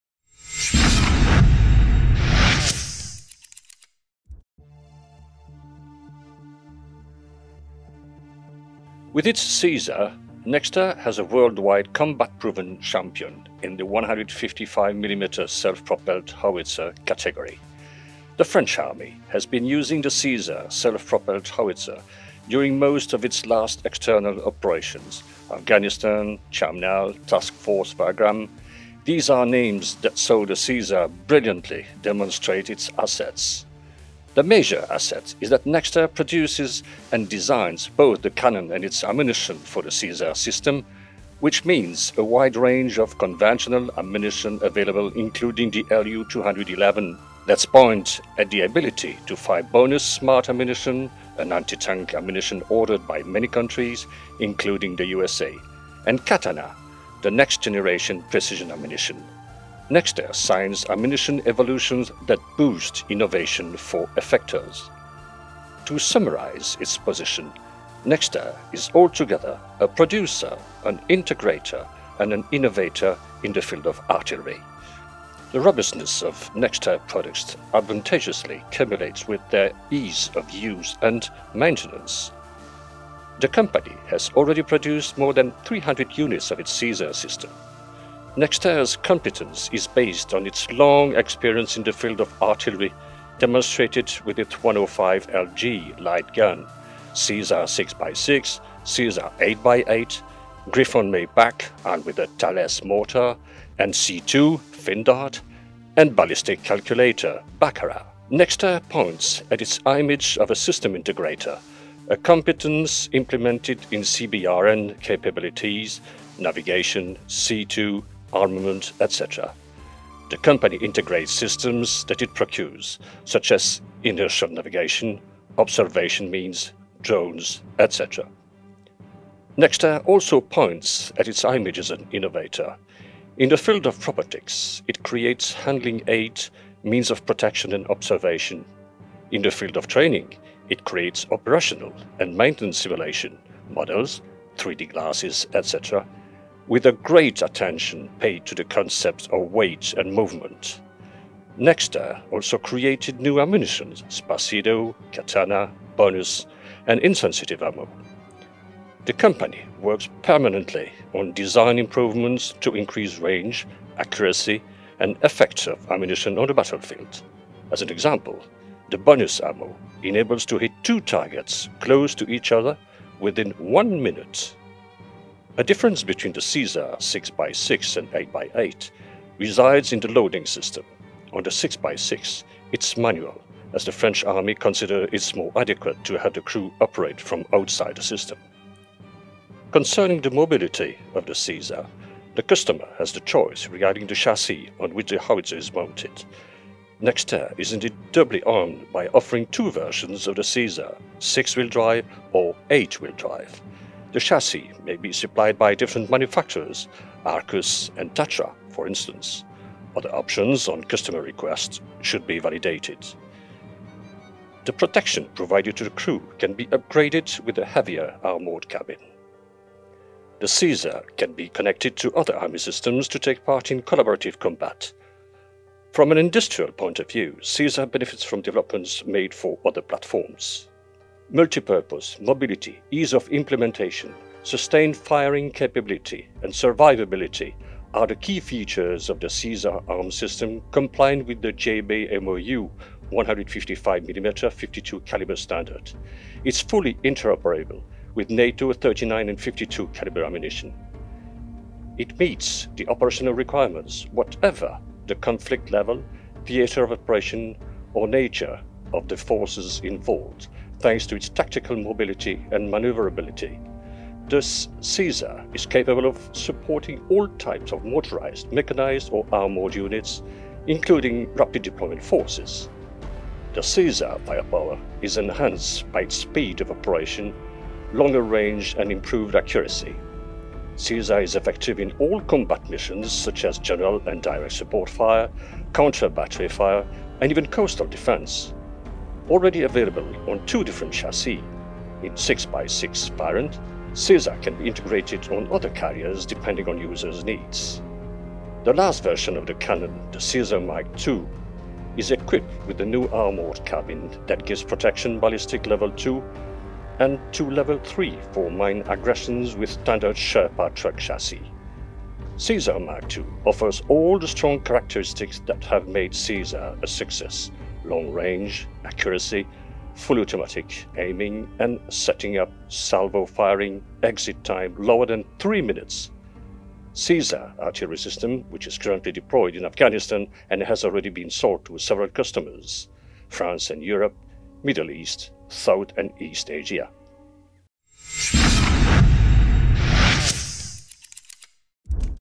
6x6 wheeled self-propelled howitzer 155m CAESAR Nexter Systems truck with artillery systems France
The French Company Nexter Systems, who is now part of the KNDS group with the German Company KMW (Krauss-Maffei Wegmann), has developed two types of wheeled self-propelled howitzers in 6x6 and 8x8 configurations. In 2008, the CAESAR 6x6 equipped massively the French Army. The CAESAR 155mm howitzer on 6x6 chassis benefits from an exceptional strategical and tactical mobility with unrivaled firepower.

Read technical review fact sheet about the 6x6 self-propelled howitzer at this